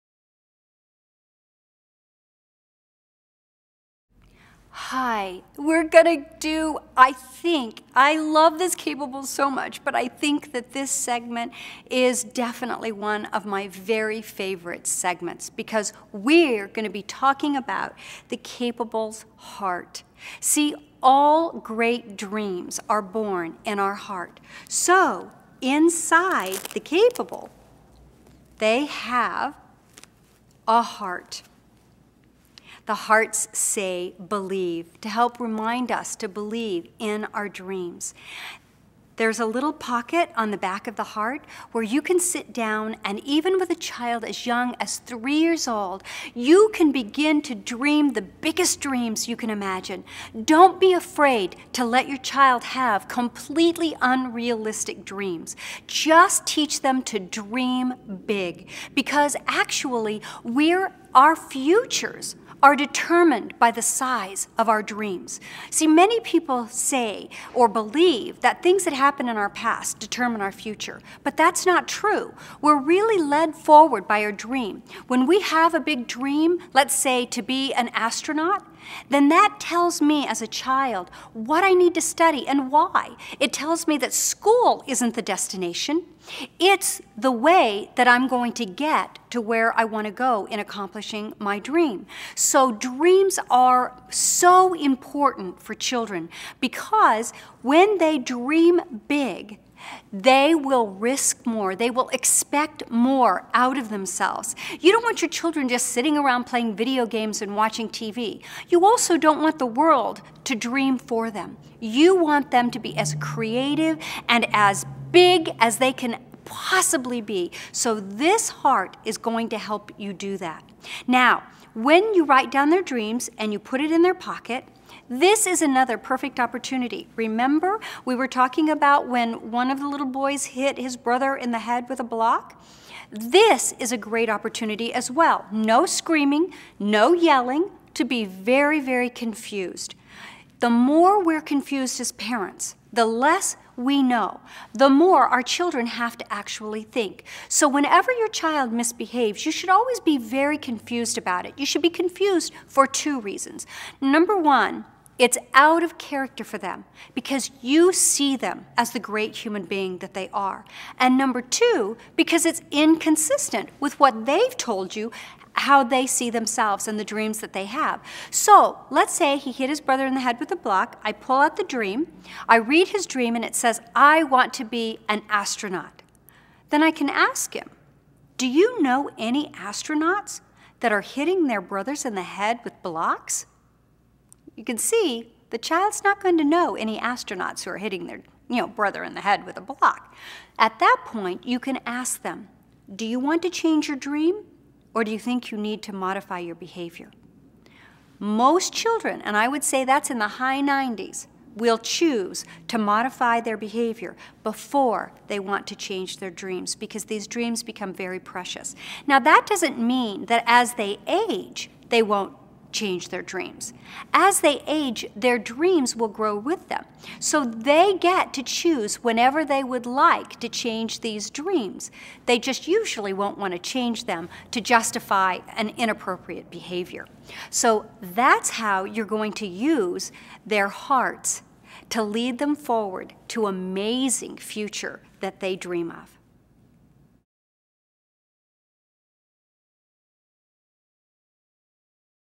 Best Parenting Videos, Dawn Billings - Capables Heart of Dreams.mov
Learn about Dawn Billings NEW Parenting Tool and Toddler Toy - CAPABLES. Dawn Billings is an author and parenting expert who has created a TOY to help you teach your children they are CAPABLE of Greatness. The Capables Early Childhood Learning System is a complete, step-by-step learning system that helps take the work out of parenting and turn it into play. This video focuses on using the CAPABLES Heart of Dreams to teach a child how to dream the biggest dreams they can imagine and to value what they love.